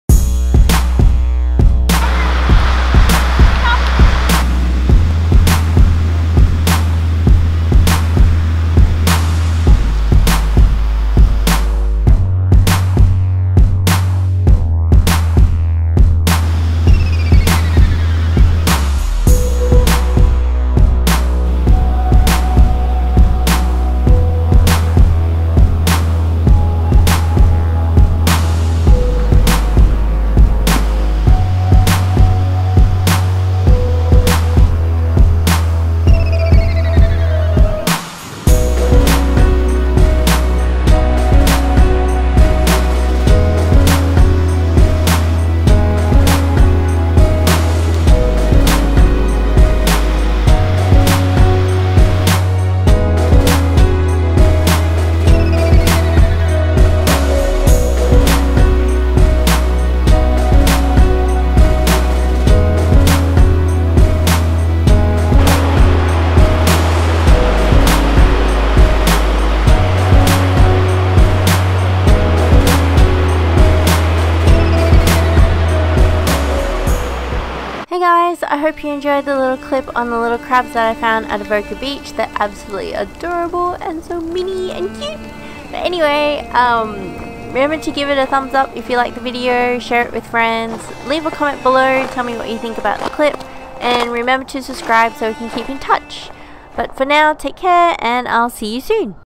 Sand Bubbler Crab - Australian Beach Crab | Music
Sand Bubbler Crabs are these small crabs that live on sandy beaches of east coast Australia. Sometimes you will see them scattering through the sand, but only a centimeter cross, these little guys feed at low tide.

They feed by putting sand grains in their mouth and filtering out the micro fauna. They disperse the sand in small round balls so they can easily roll them aside. Generally they work fast because they can only feed at low tide, when the sand is damp. But can clean up to one meter of the sand surface above its burrow, this usually takes them a few hours. Once they have completed the task they will return to their burrow and wait for the next low tide.

Narrator: Sandy Wong

Being an Environmentalist I want to share with you the places I go and the wildlife I see. So come walk with me and experience our world.